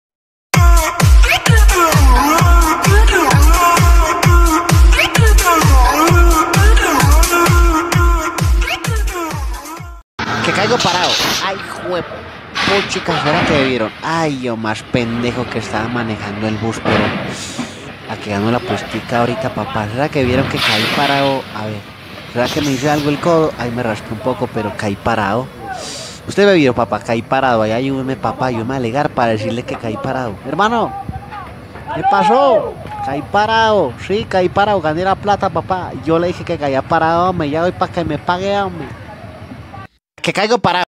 Accidentes en carretera
Video que contiene un accidente en carretera por andar apostando